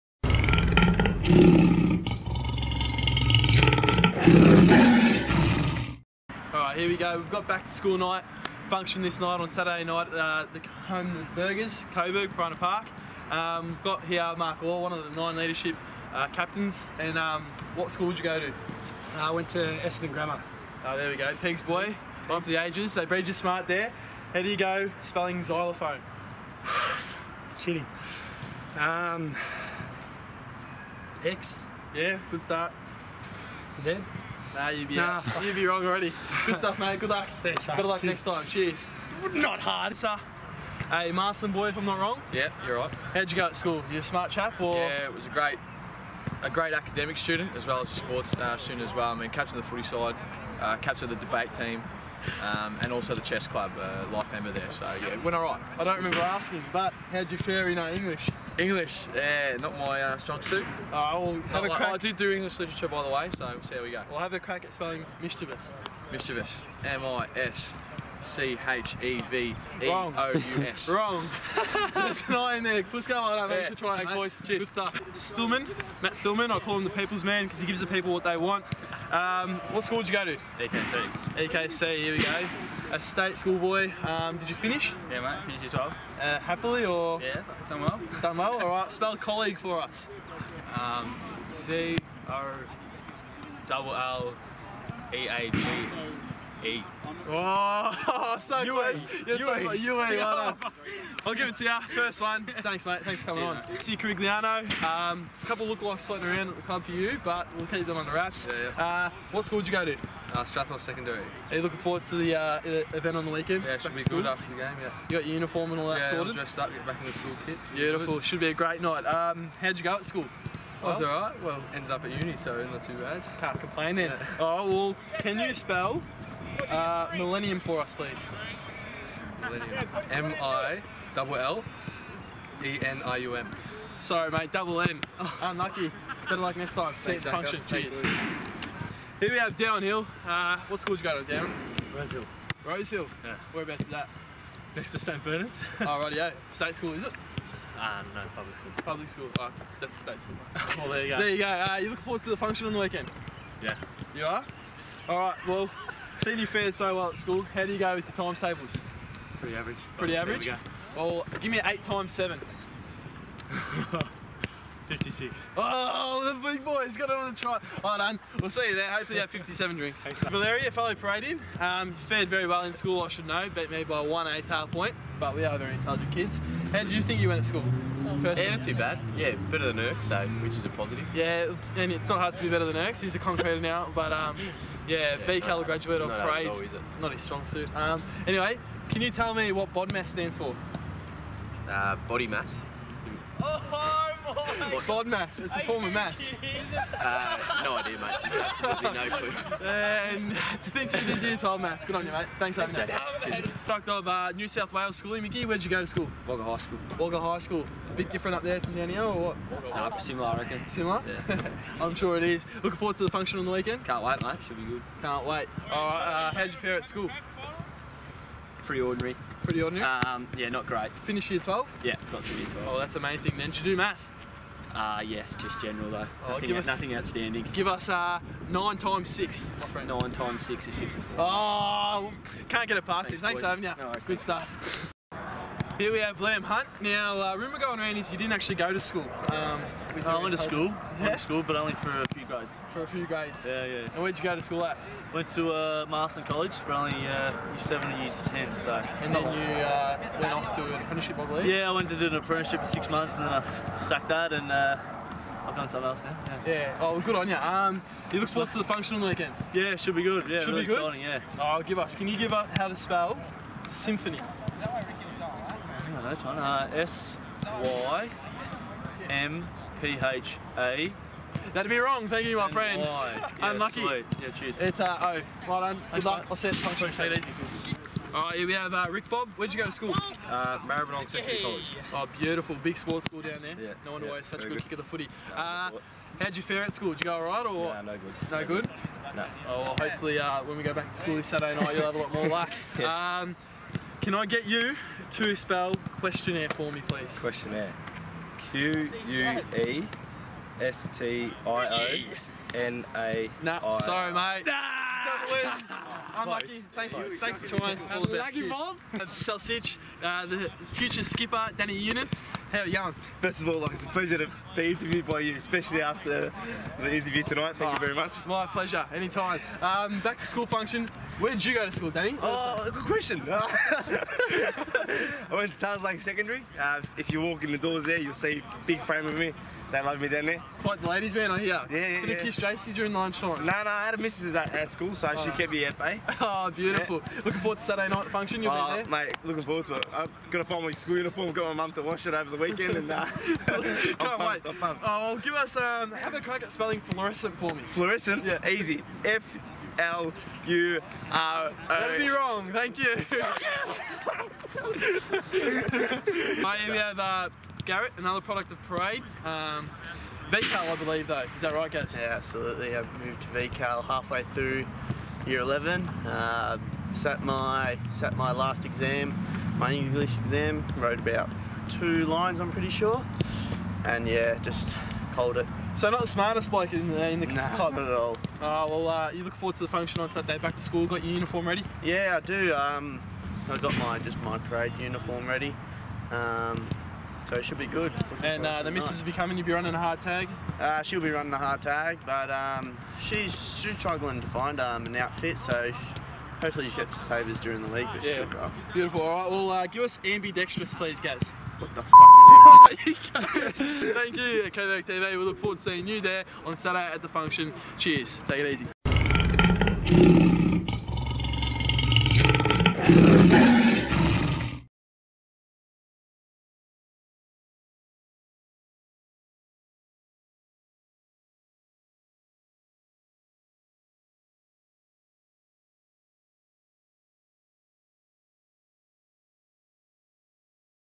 Coburg TV - Episode 2, 2016
With the boys going 'back to school' at a club function in past weeks, we thought we'd take a trip back to the high school days on Coburg TV, to test players on some spelling and mathematical problems!